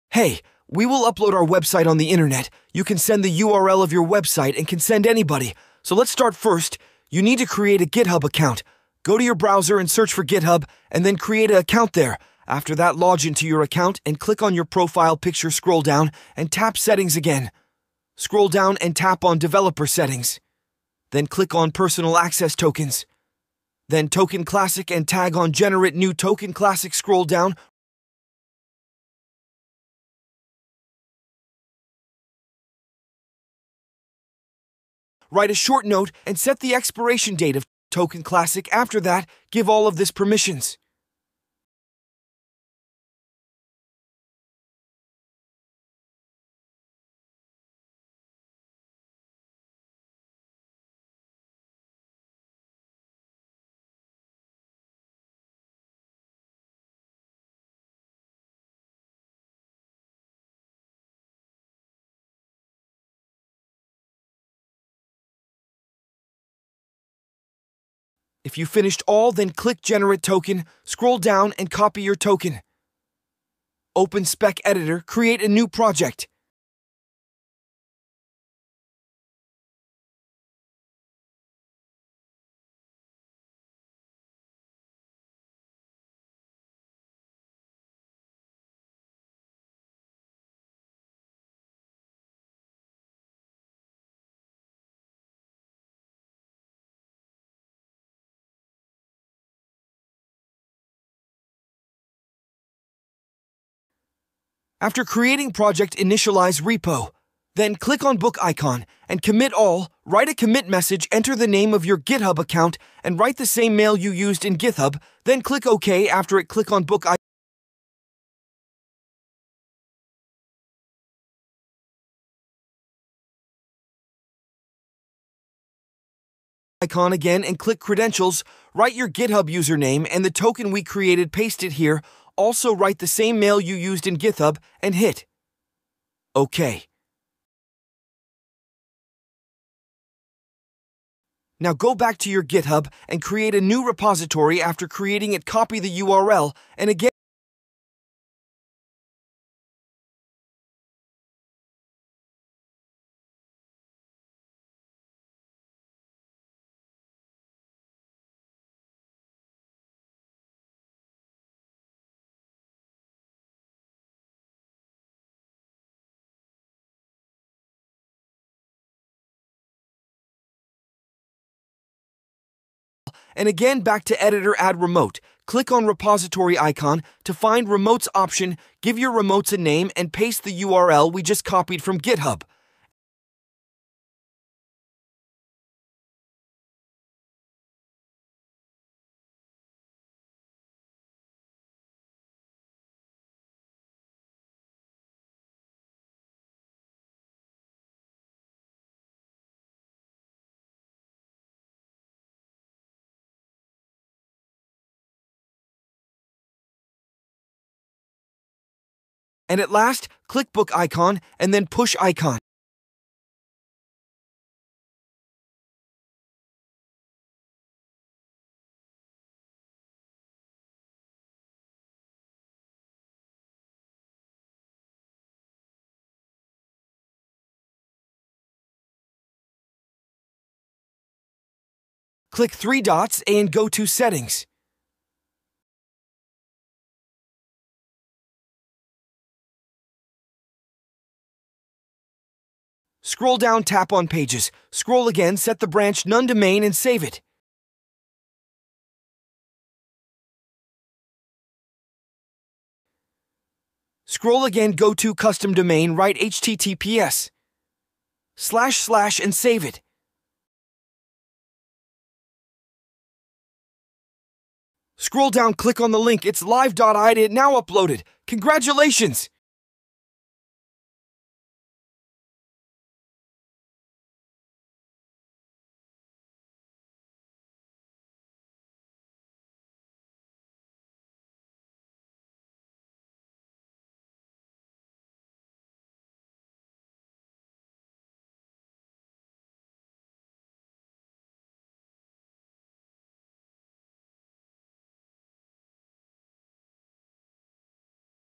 How to Upload Your Website on the internet and Make It Live with your phone!
In this video, we'll walk you through the entire process of uploading your website to GitHub and making it live using GitHub Pages. From creating a GitHub account and setting up a personal access token to using Speck editor for managing your project, we cover every step in detail. Follow along as we show you how to create a repository, commit your code, set up your GitHub Pages, and finally launch your website for the world to see. Perfect for beginners and web developers looking to deploy their projects easily and for free.